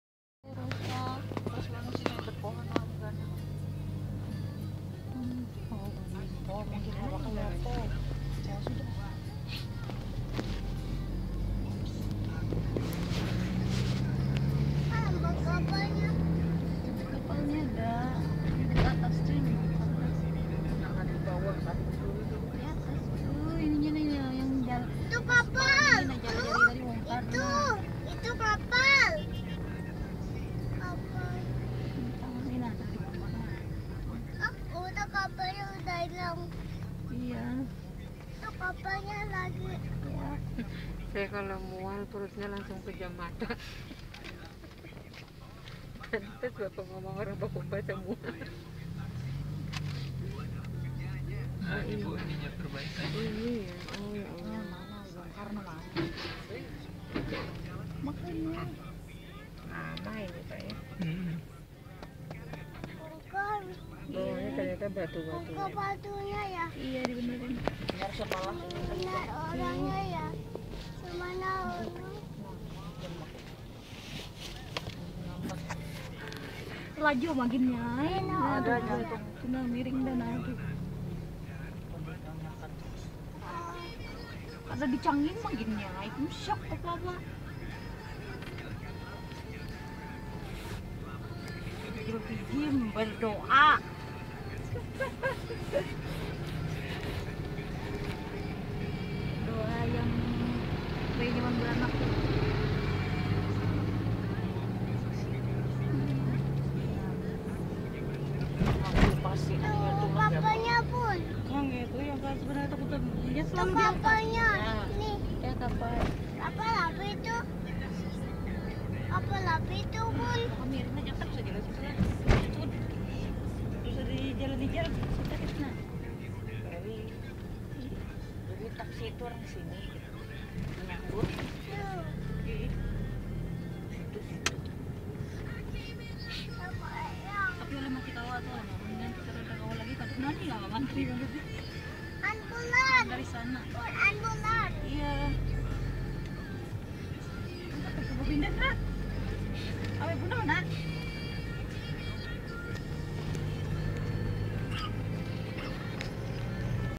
Perbaikan Jembatan Pulau Telo Kuala Kapuas 2017
INFORMASI KAPUAS - video ini direkam pada hari Sabtu, 4 November 2017 saat menuju ke Palangka Raya.